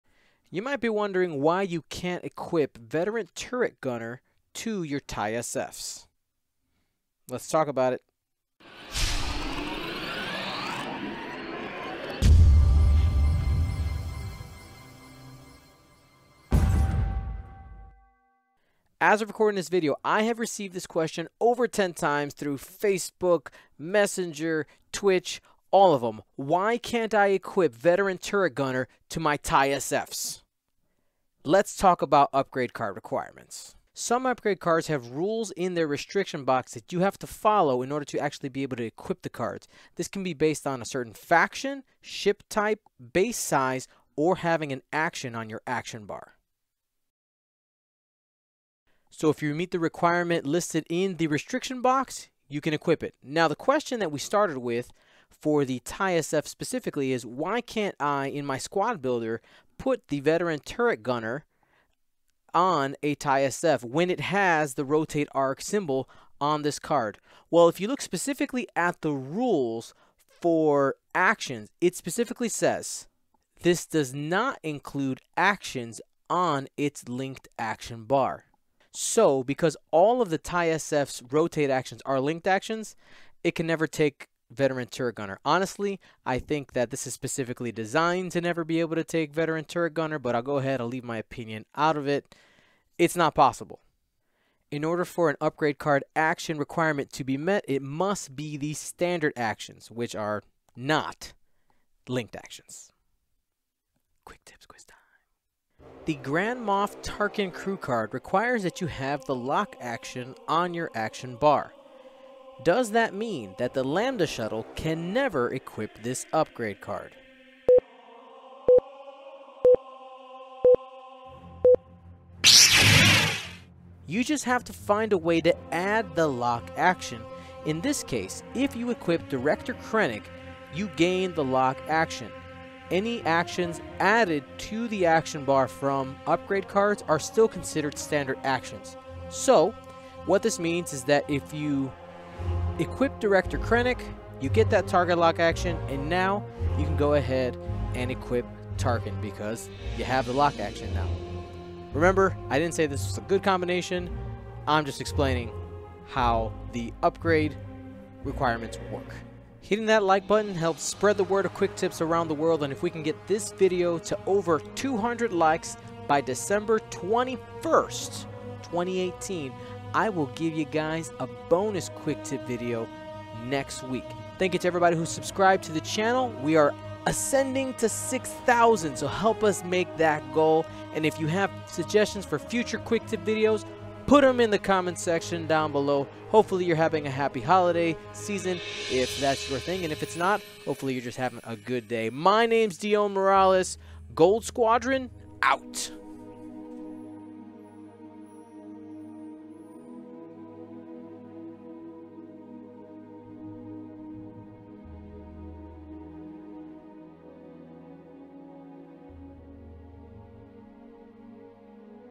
Why Can't I Equip Vet Turret Gunner to the Tie/SF??? - Upgrade Requirements -X-wing Quick Tips Ep. 9
Useful tidbit of X-wing information in 5 min or less, Watch Gold Squadron X-wing Quick tips every Monday!